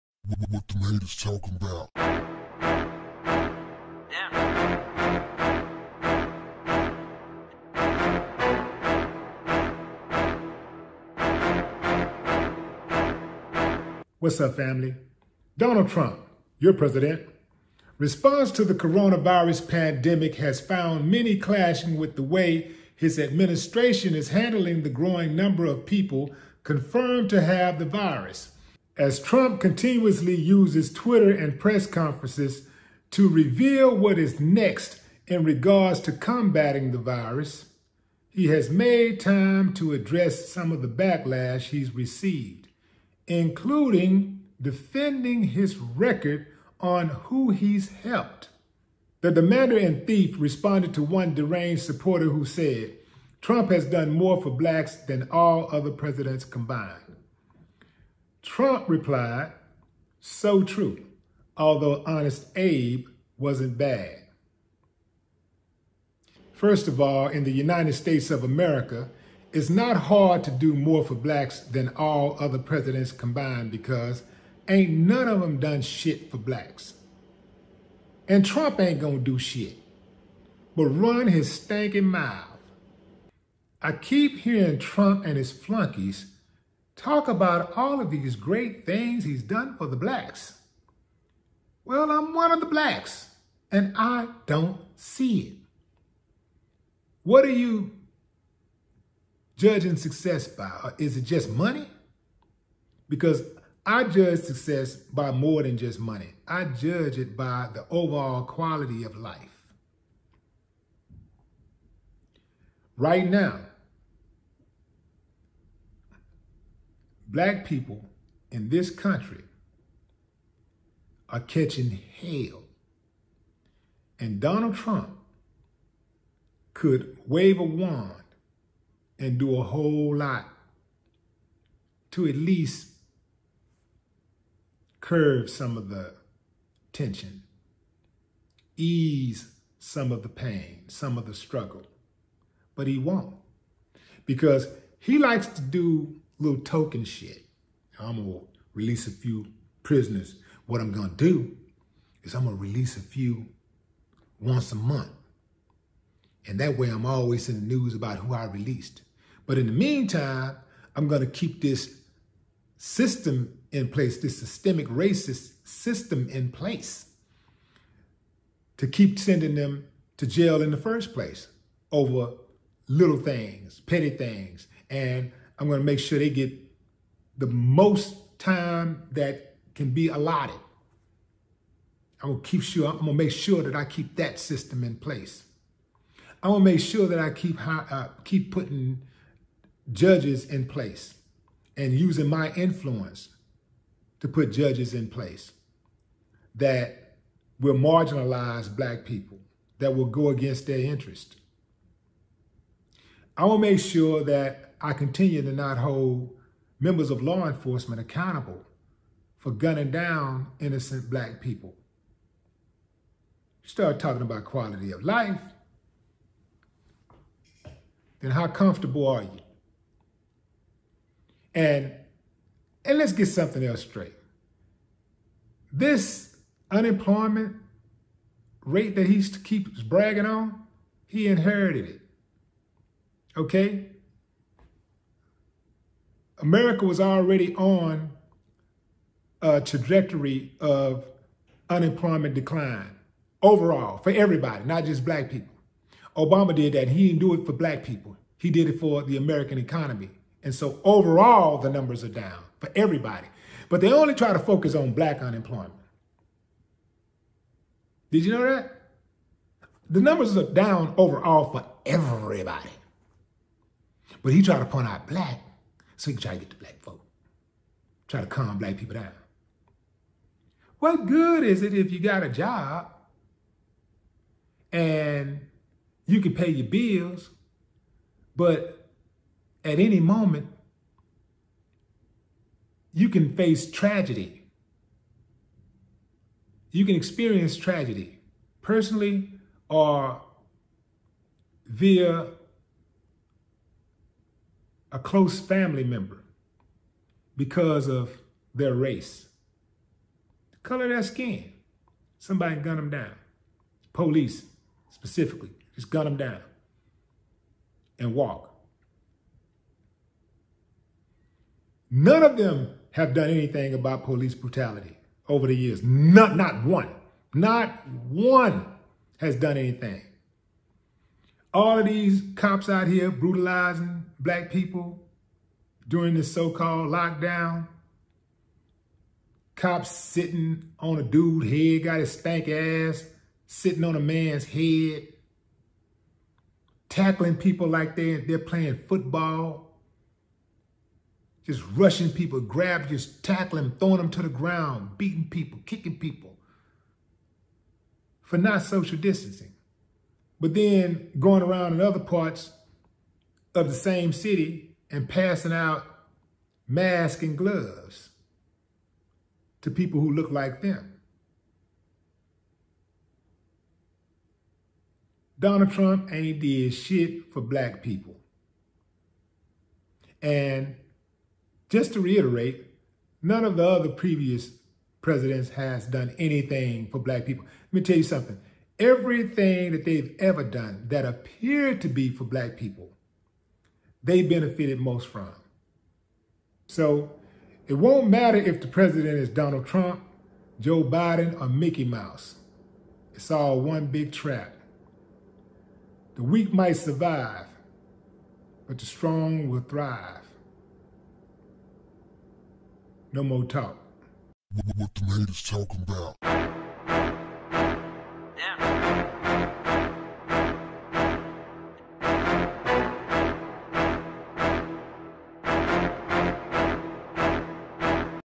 Trump Retweets Claim He "Did More for Blacks Than All Other Presidents Combined"
$WillieDLive